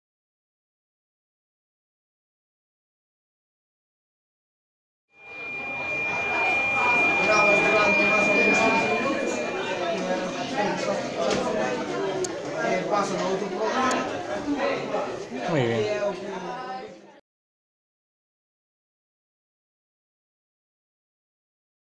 Pantalla lámpada Litofanías
Proxecto Día da Ciencia en Galego no IES. Chano Piñeiro. Pantalla de lámpada realizada con 4 Litofanías.